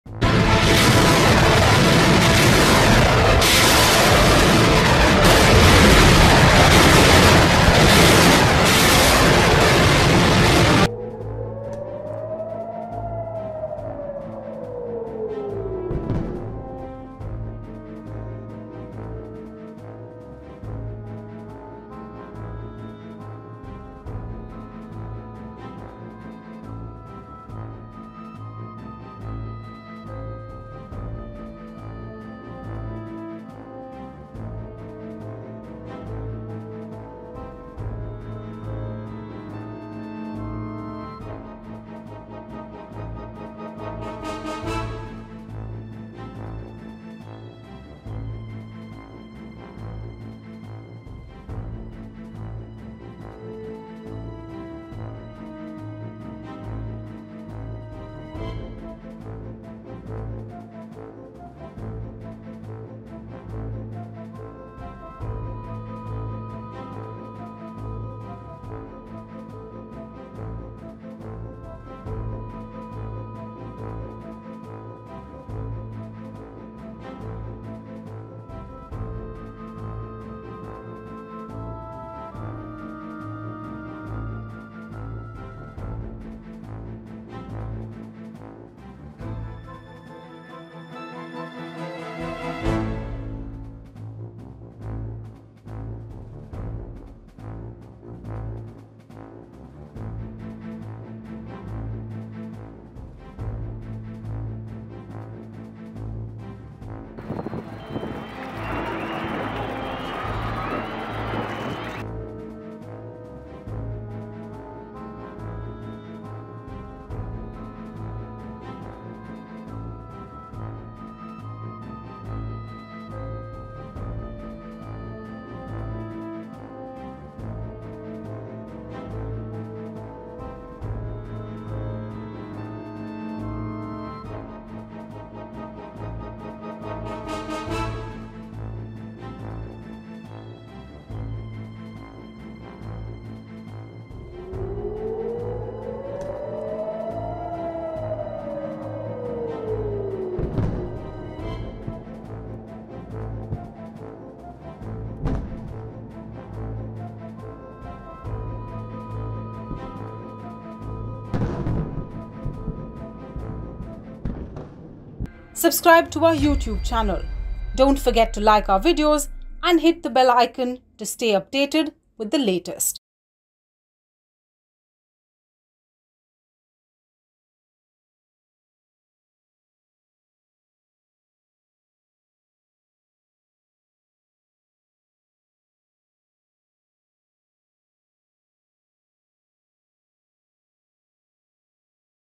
'Haniyeh Killing Great Crime': IRGC Commander Vows Harsher Revenge Than April Attack On Israel
Iran’s Islamic Revolution Guards Corps (IRGC) has vowed that it will avenge Hamas leader Ismail Haniyeh’s assassination but the time and place will be decided by Tehran only. IRGC Deputy Commander Brigadier General Ali Fadavi said Israel will be punished more severely for the great crime of murdering Ismail Haniyeh in Tehran. Watch.